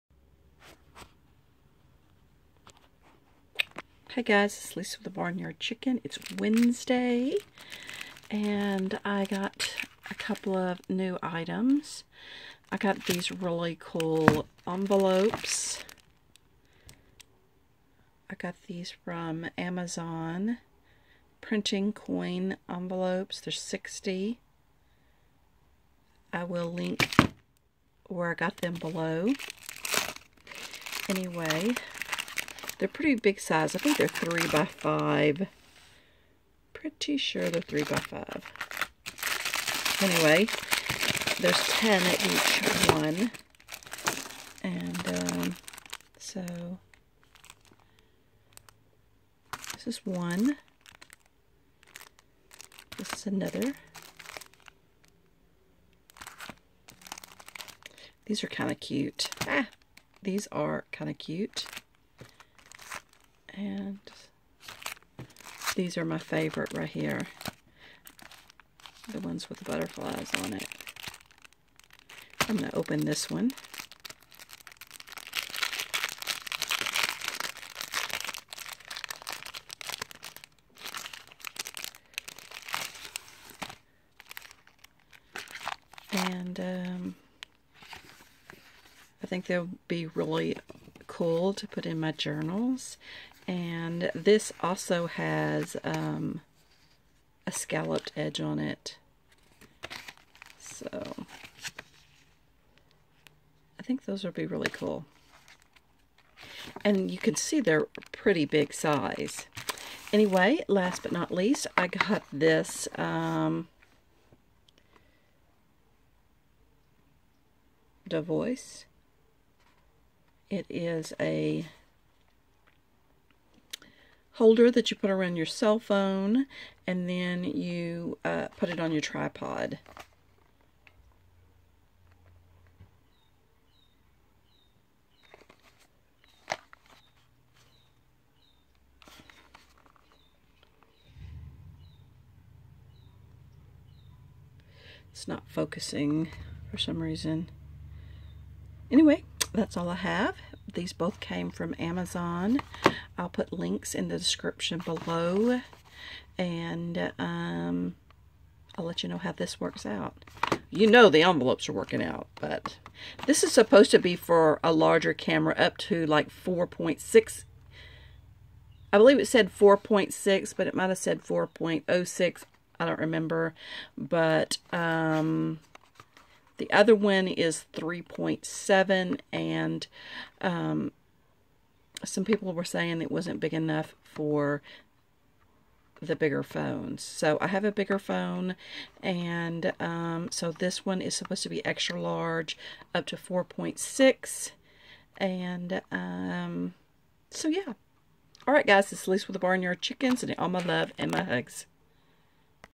Small Amazon Haul
A couple of new items I picked up some coin envelopes for my scrap journaling and a bracket for my phone

If you like the envelopes I got them

DaVoice Phone Bracket
Ad says it fits up to 4.06" wide phones